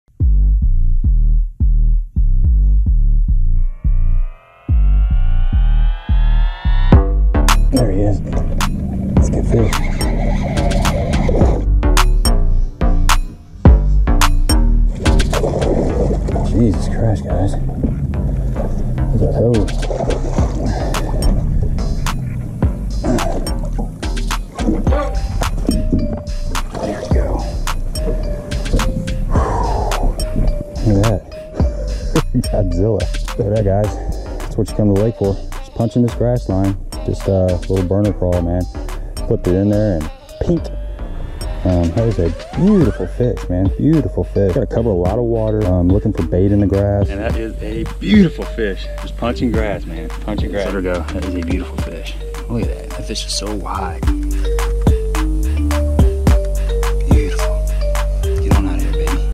Monster BASS Punching Grass Mike Tyson Style
Fall is a perfect time to punch grass and one of my favorite baits for punching grass Mike Tyson style is a Gambler Lures Burner Craw.

Shop Bait-WrX for the best prices and fast shipping.